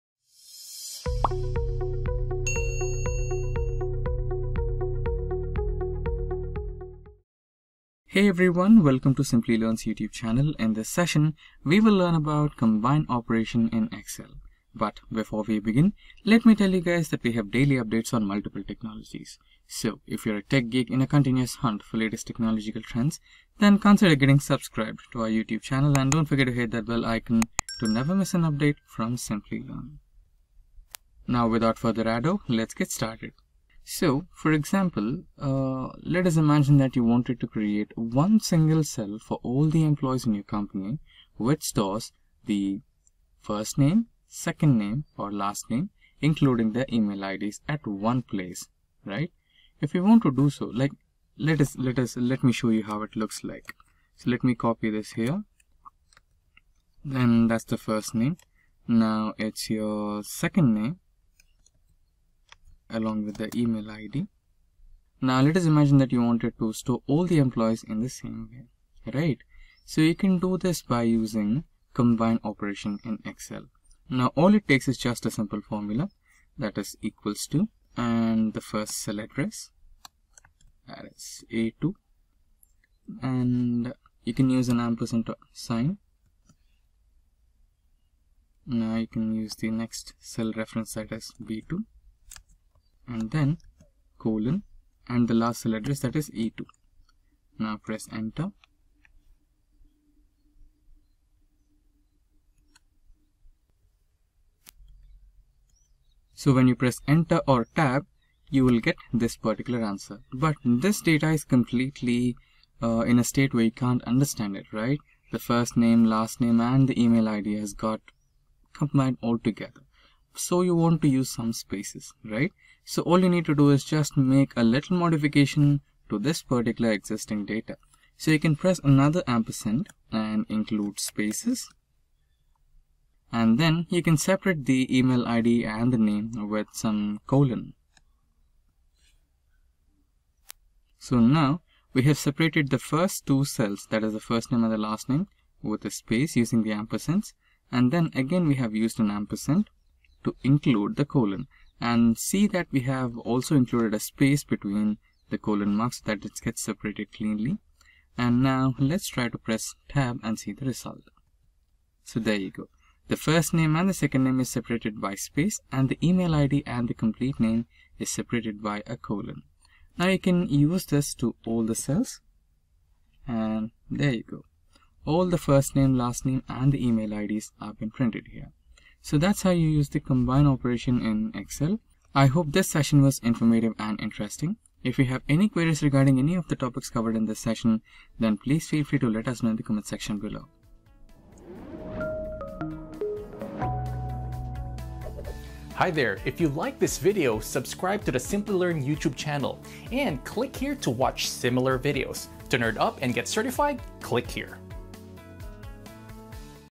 Combining Data From Multiple Cells In Excel | How To Combine Data In Excel | Simplilearn
️️🔥 Professional Certificate in Data Analytics and Generative AI, by Simplilearn in collaboration with Purdue University

This video is based on Combining Data From Multiple Cells In Excel. Combining data in excel comes handy when you want to create a considered profile type cell that includes all the major information at one place. For example, employee name, ID and mailing address. This tutorial will guide you in a step-by-step fashion where you learn how to combine data in Excel.

00:00 Introduction to Combining Data From Multiple Cells In Excel.

This section will give you a detailed information on the process of Combining Data From Multiple Cells In Excel.

How to Combine Data in Excel
Here, in this section of Combining Data From Multiple Cells In Excel tutorial, you will be able to practically implement the process of combining data in excel.

What is Excel?
Microsoft Excel is a spreadsheet program. That means it's used to create grids of text, numbers and formulas specifying calculations. That's extremely valuable for many businesses, which use it to record expenditures and income, plan budgets, chart data and succinctly present fiscal results

How to Combine Cells in Excel?
Combine text from two or more cells into one cell
Select the cell where you want to put the combined data.
Type = and select the first cell you want to combine.
Type & and use quotation marks with a space enclosed.
Select the next cell you want to combine and press enter. An example formula might be =A2&&B2.

✅ Skills Covered
- Data Analytics
- Supervised Learning
- Unsupervised Learning